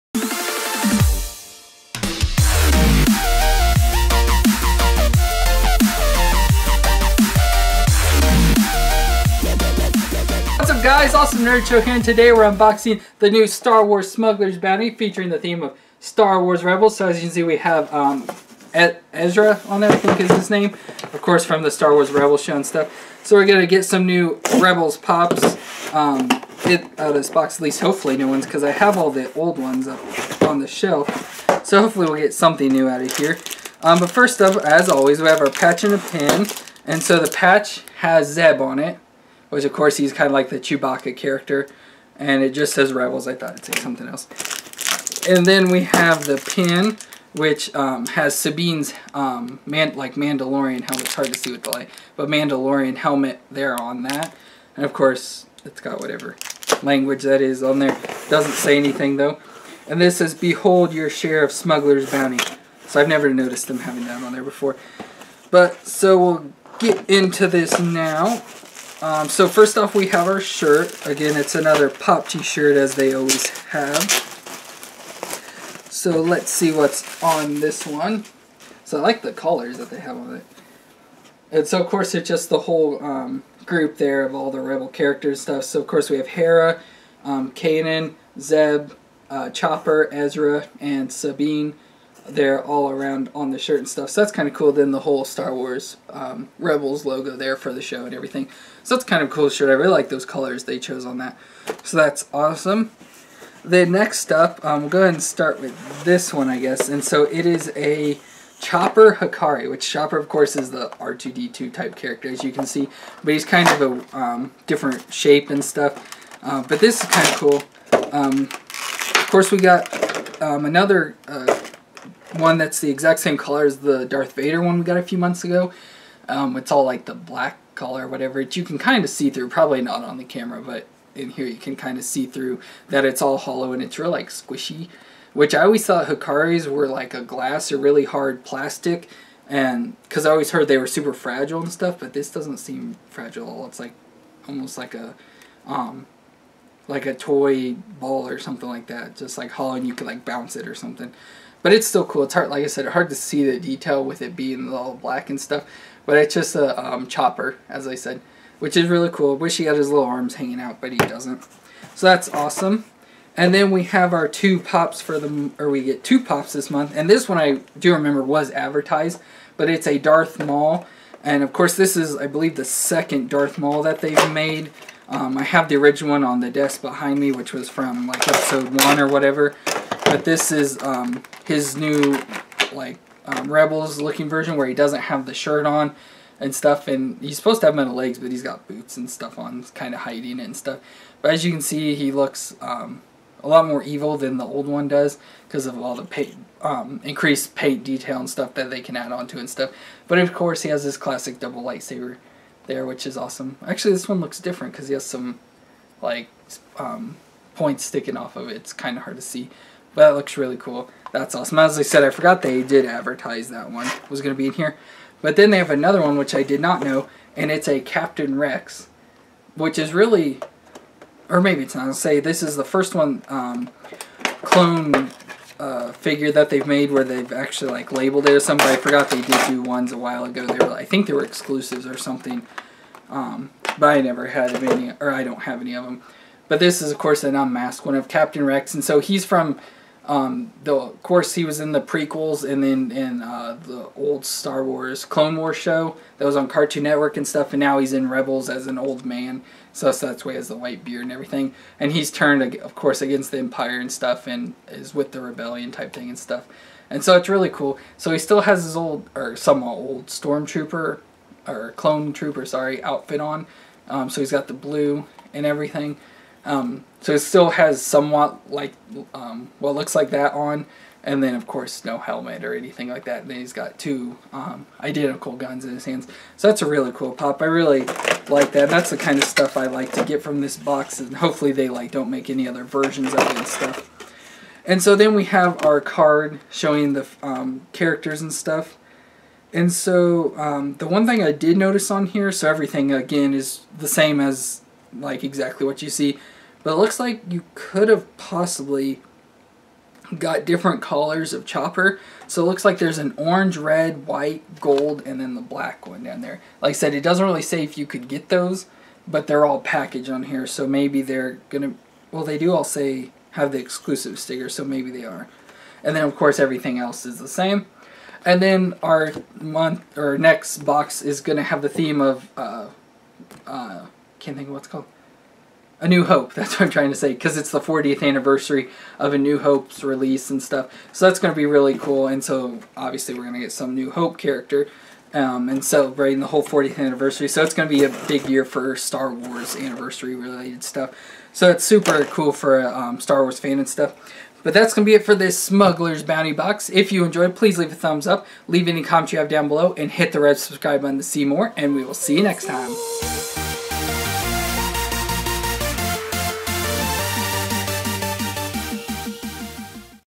Star Wars Smuggler's Bounty - Star Wars Rebels
Unboxing this months Star Wars Smuggler's Bounty box powered by Funko and features the theme of Star Wars Rebels!